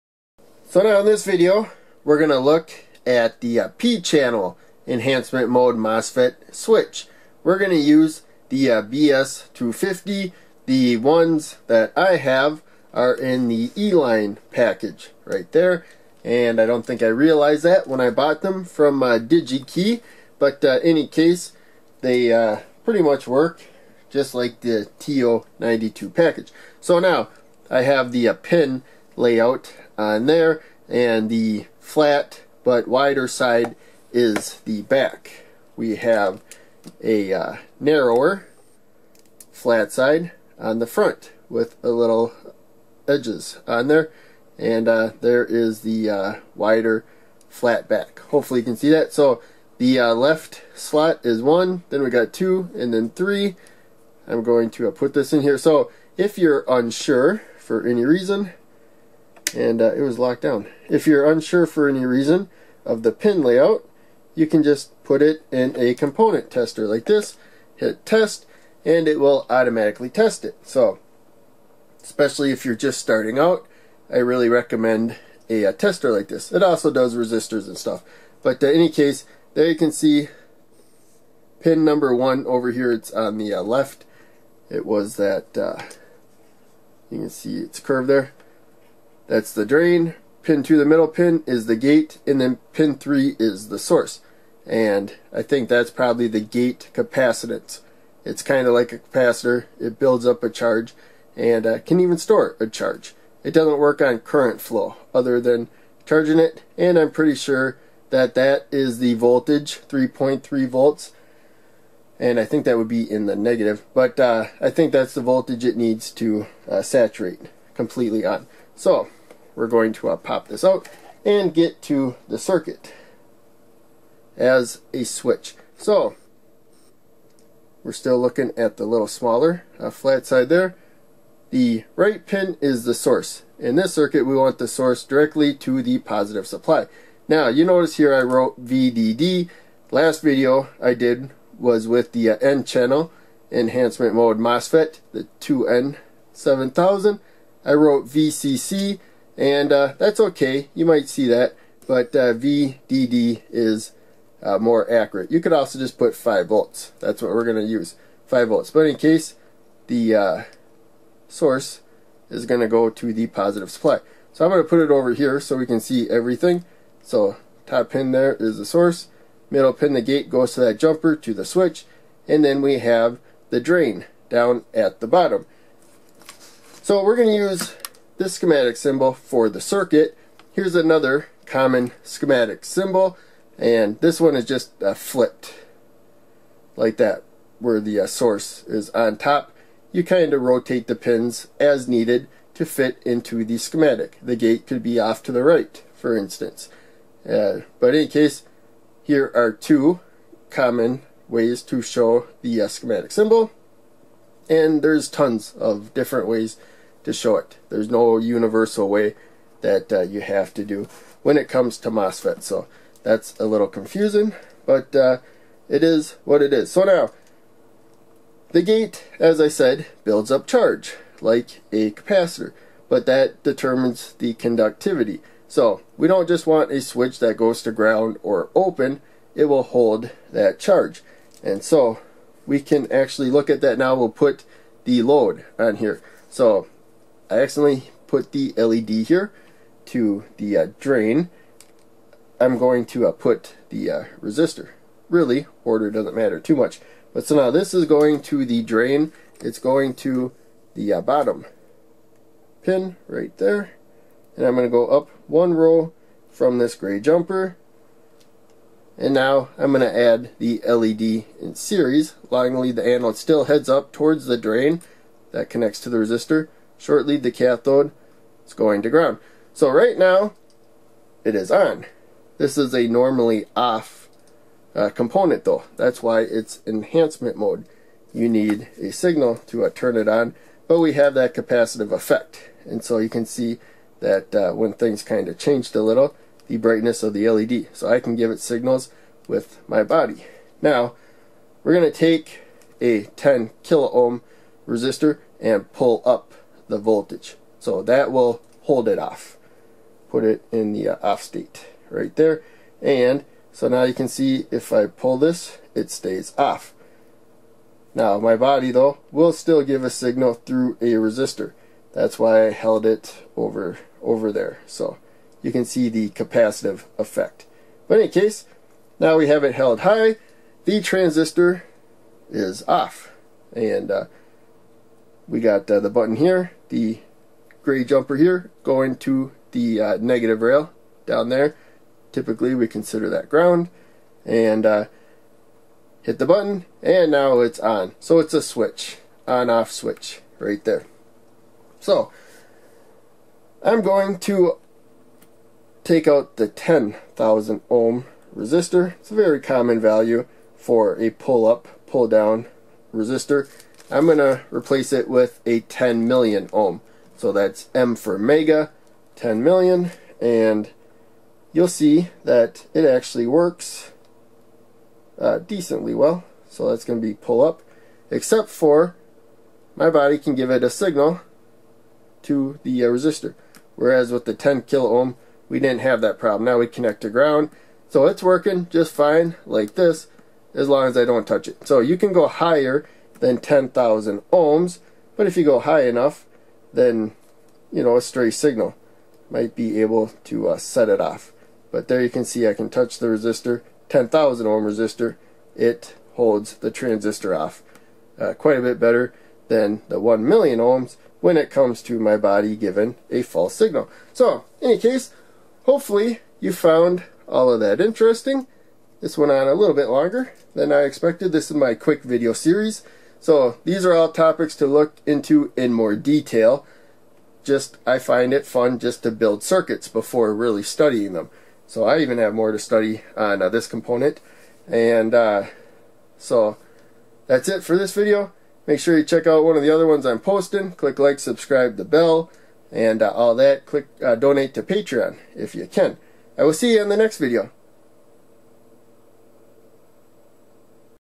Quick BS250 E line package P channel enhancement mode MOSFET switch circuit by electronzap
BS250 is a P channel enhancement mode MOSFET. It makes a good voltage control switch. It does not depend on gate current for how well it conducts from source to drain. It is off when the gate is to the positive rail and on when the gate is to the negative rail. A pull up or pull down resistor can be used to prevent the gate from ever floating, and holding a charge.

Electronics quick video series tutorials by Electronzap and Electronzapdotcom.